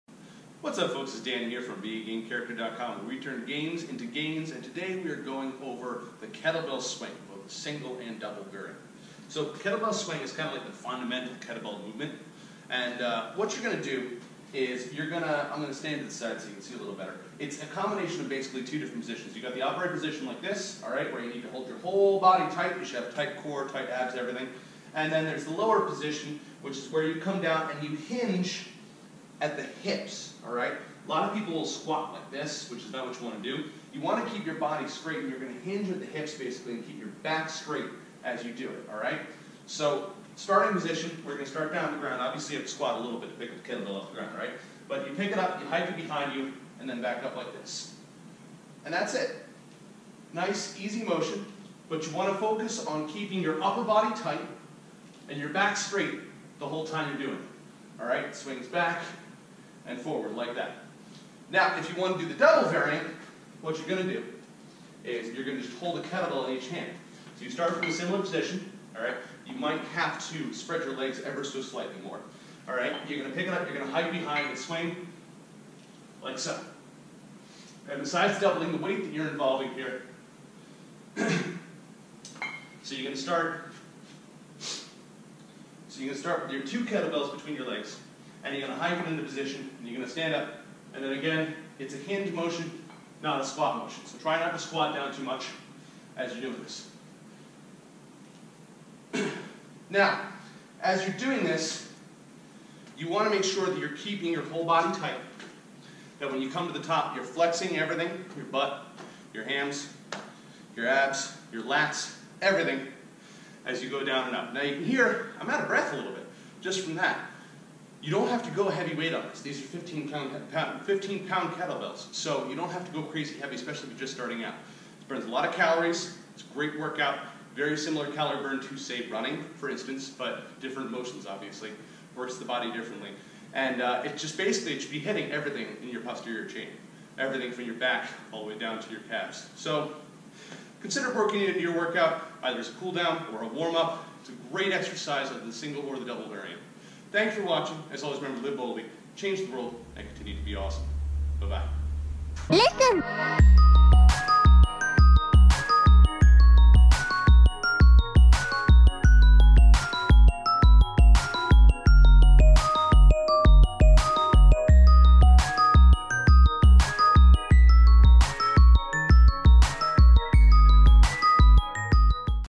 How to Perform a Kettlebell Swing (Single and Double Variants) - Exercise Tutorial
The kettlebell swing is the iconic total-body kettlebell exercise, and works your entire posterior chain.